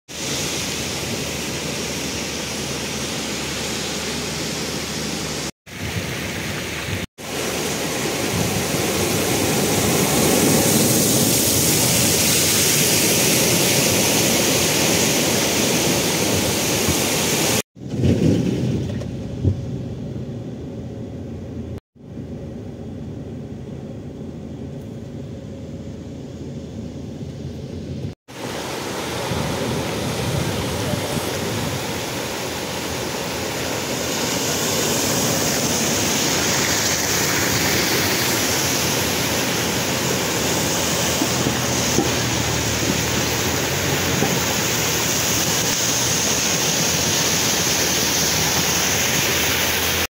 Dio Vukovarske ulice u Makarskoj poplavljen nakon obilne kiše
VIDEO: Matko Begović/PIXSELL